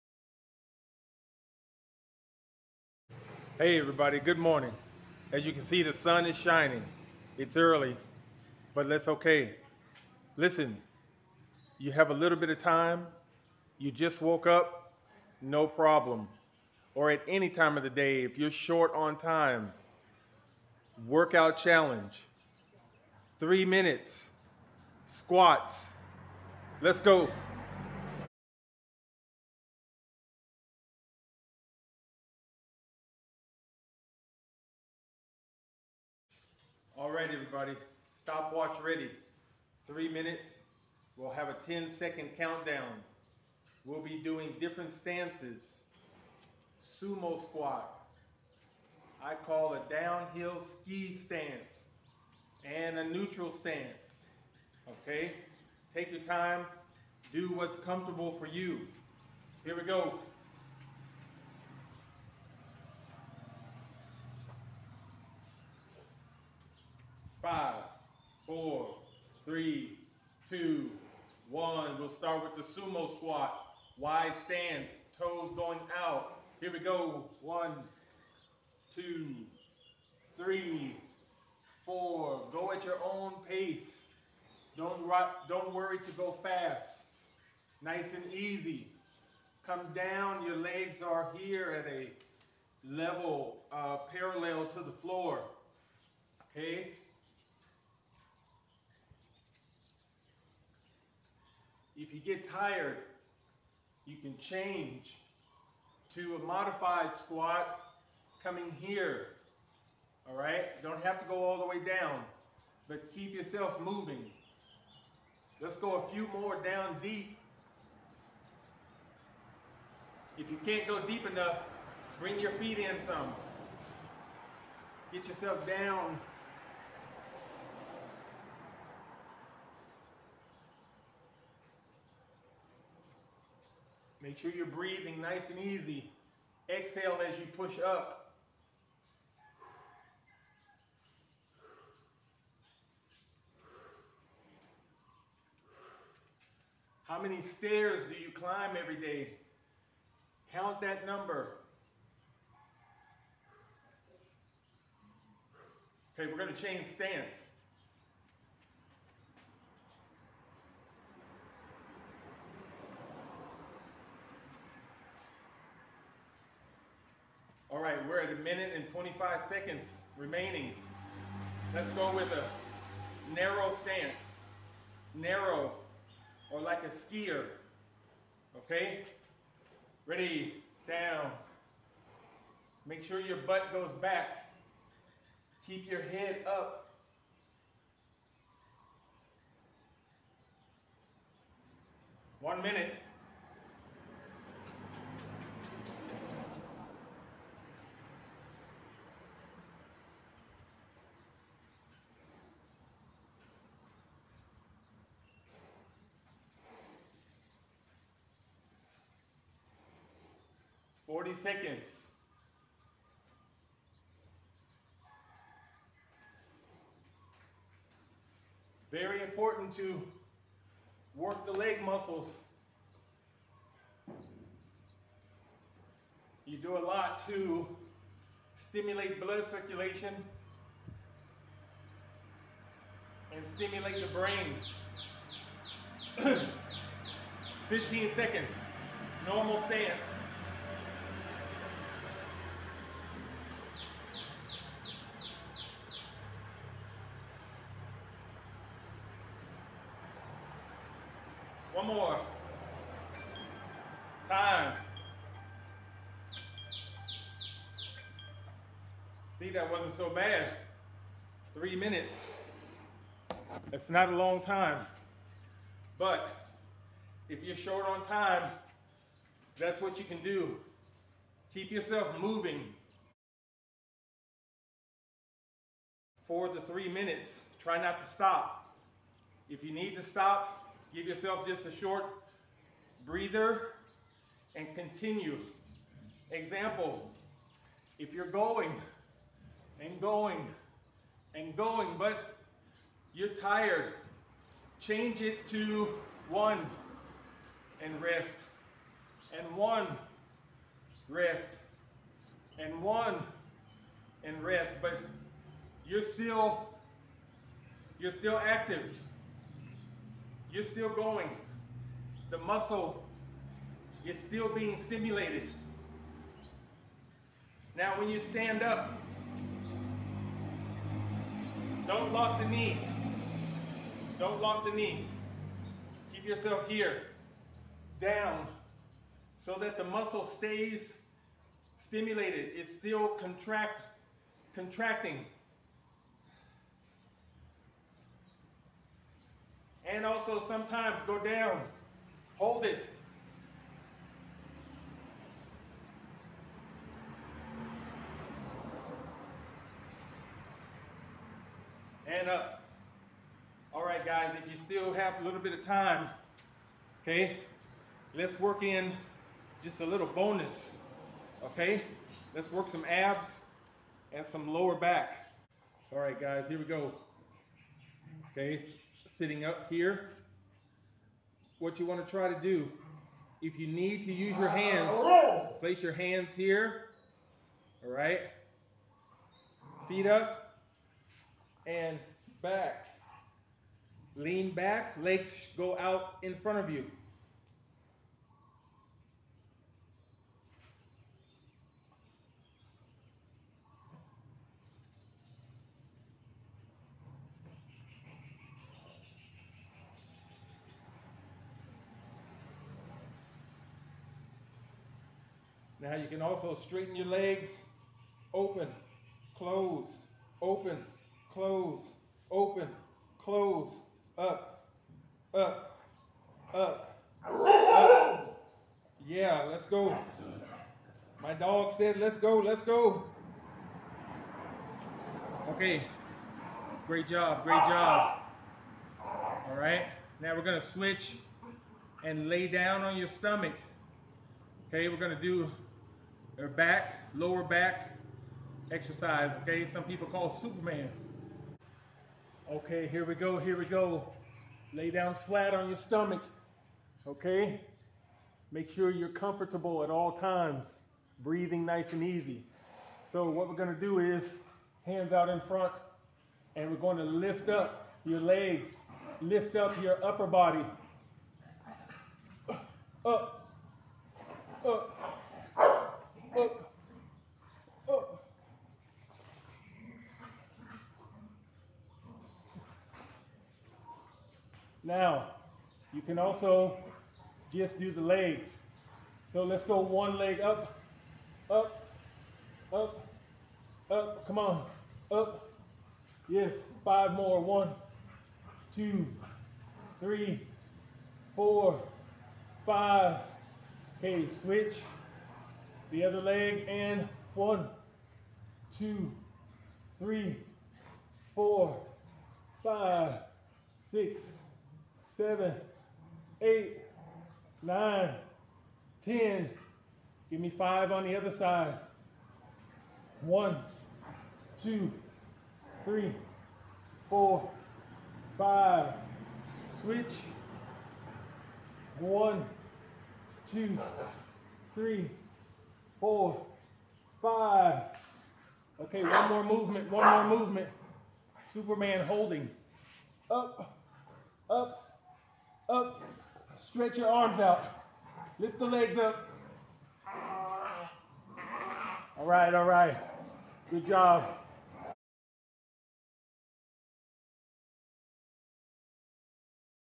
Fitness Challenge 3 min #2- Squats bodhifit bodhiacademy. Great Legs
Great satisfaction in this workout. For beginners to advanced fitness enthusiasts, when you are short on time and want to and need to do something. Different stances can be used, sometimes I stick to one stance and use the other stances for future training sessions, 'do what you like'.
My performance in this video is set for beginners to follow, advanced individuals keep it going.